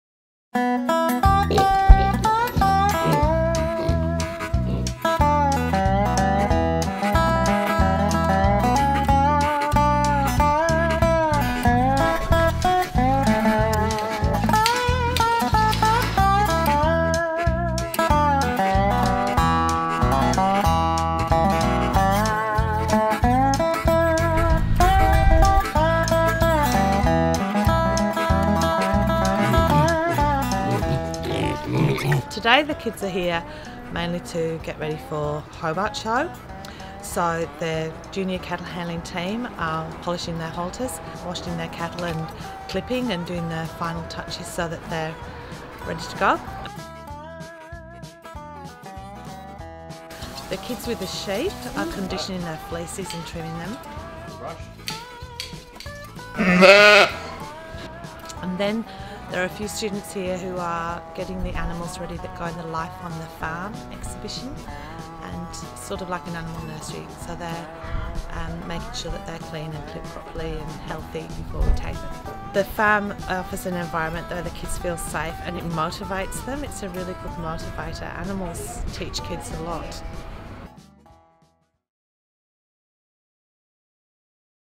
It's showtime! Students prepare for the Royal Hobart Show
How do you wash a cow? Comb a sheep? Placate an alpaca? The students of the Jordan River farm use all these skills in their prep for the Royal Hobart show.